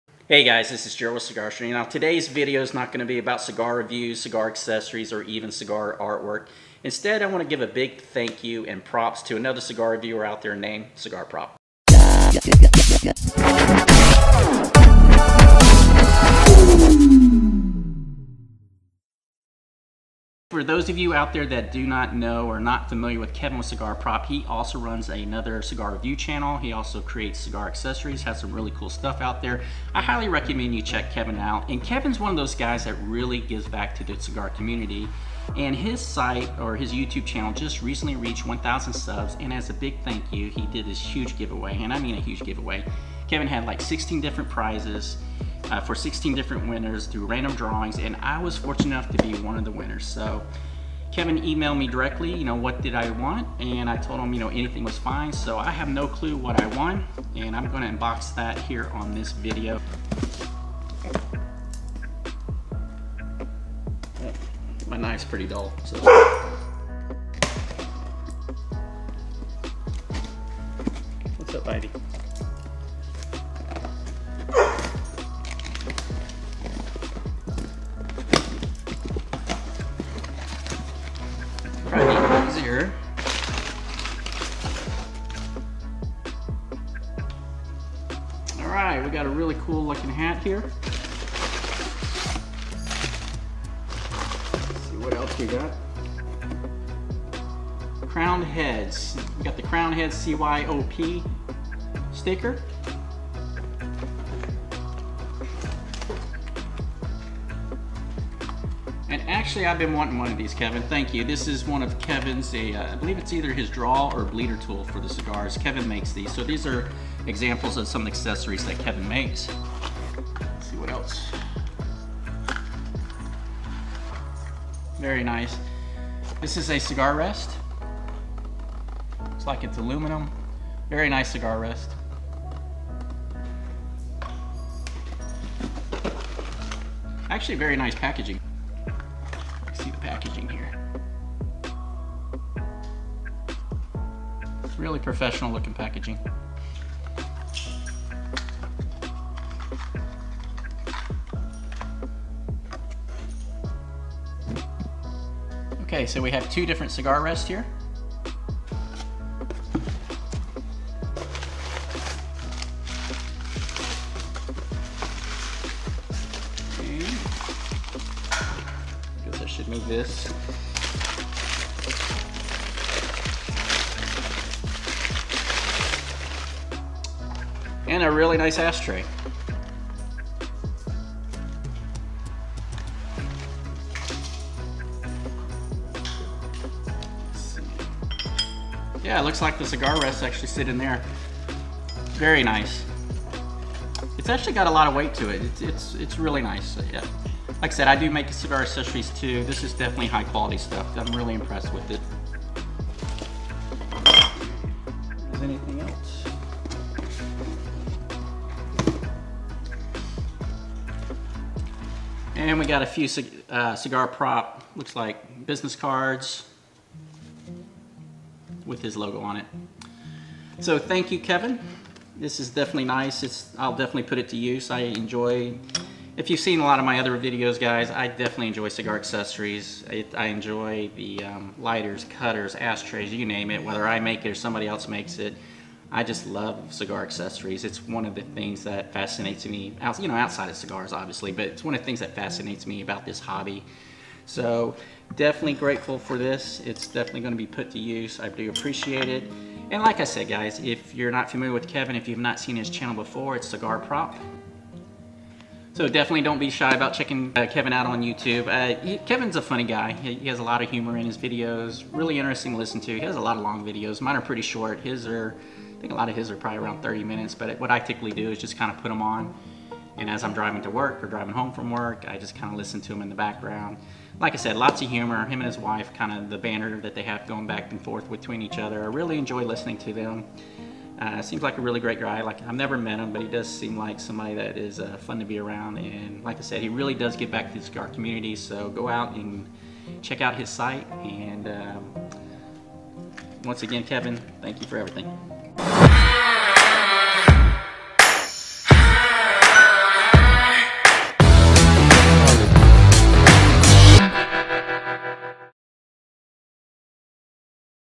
Giving Props to Cigar Prop!
I want to give a special thanks to Kevin Shahan with the YouTube channel Cigar Prop. I was a recent contest winner and in this video I go through an unboxing of my winnings.

Want to see more of my cigar artwork? You can view more on the following sites: